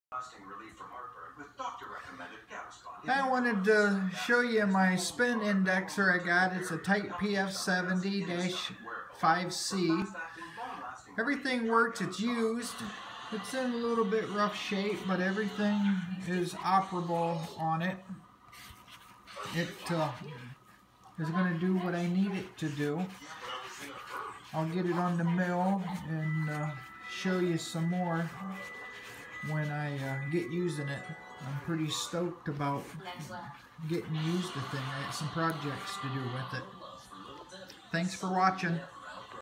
Spin index
Review of my new to me spin index